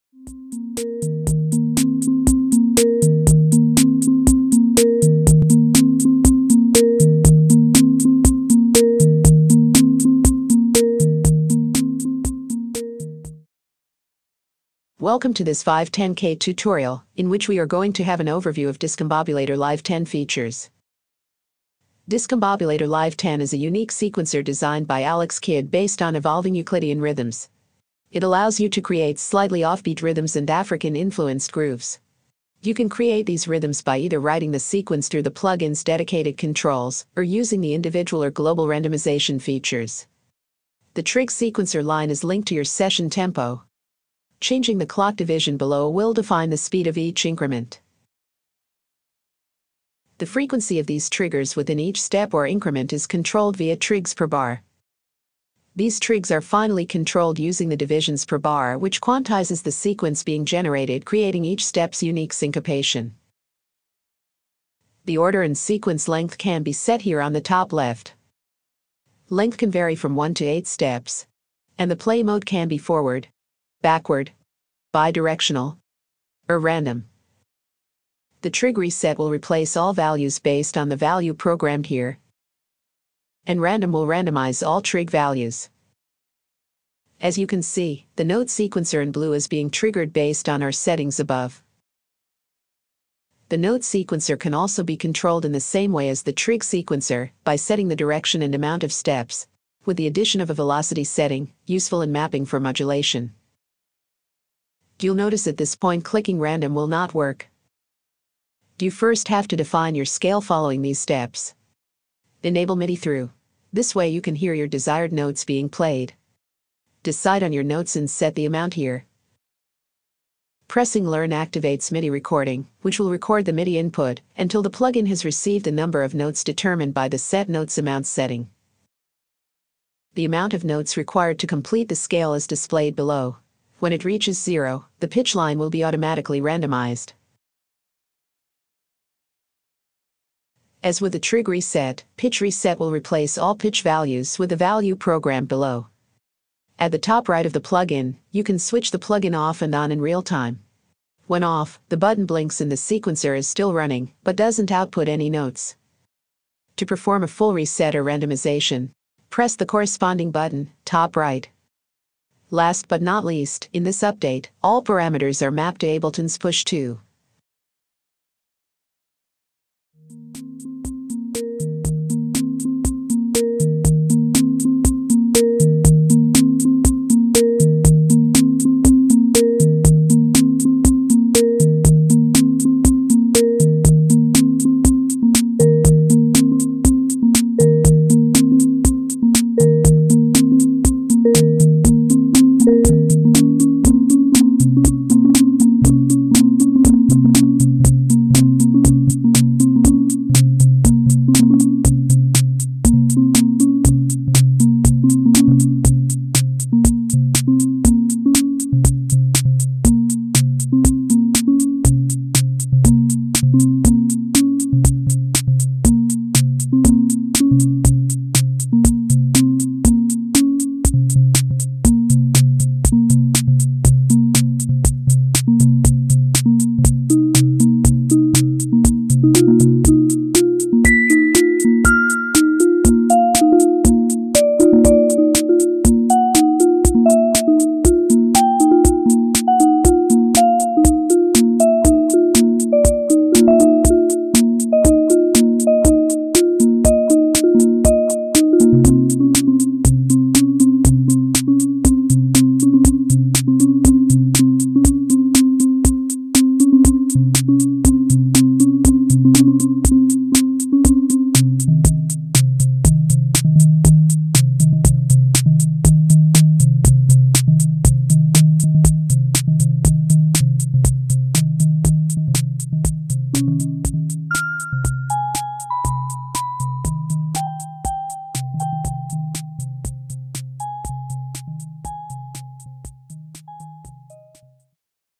“Discombobulator Live10" by Alexkid - Ableton Sequencer Tutorial & Demo
Video Tutorial for the “Discombobulator Live10" plug-in by Alexkid for Ableton / Max4Live